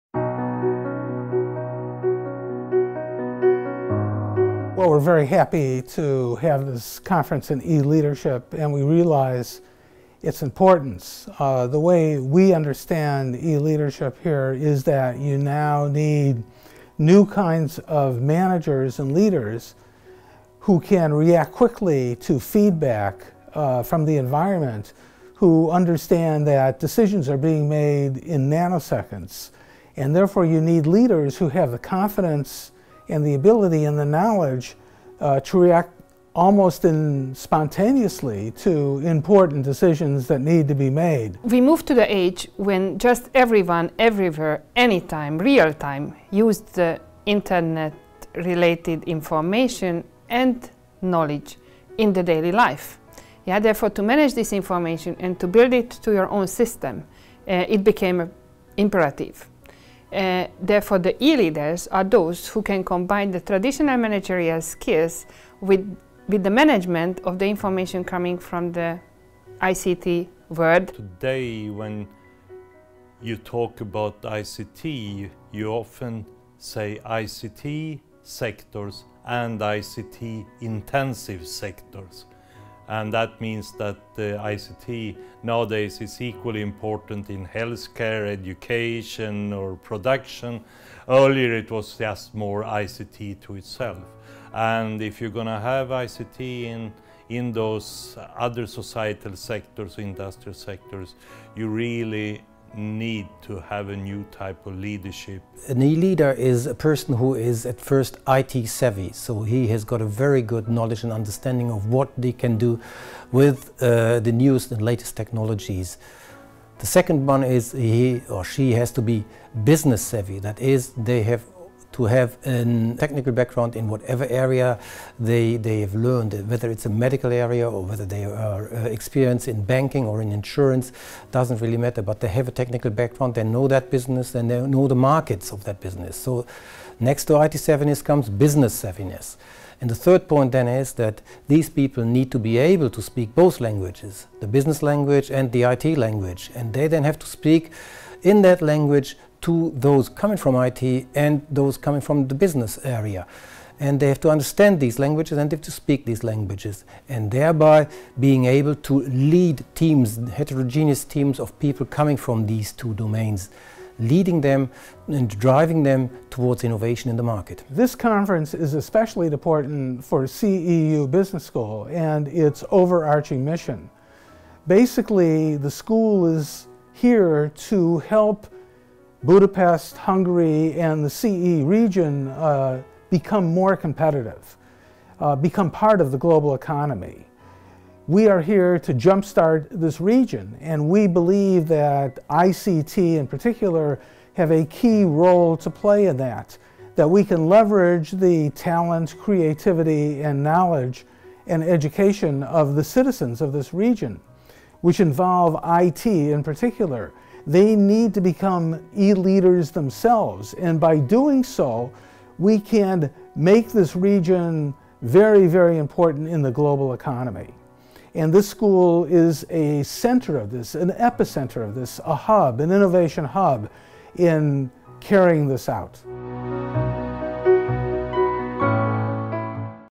What is E-Leadership?
E-Leadership is of crucial importance for companies and industry to excel in their business operation. For effective E-leadership, the skills required are seen as those which enable people with very strong Information and Communication Technology (ICT) skills to lead qualified staff from ICT and other disciplines towards identifying and designing business models and exploiting key innovation opportunities. Their success is defined as making best use of developments in ICT and delivering value to their organizations.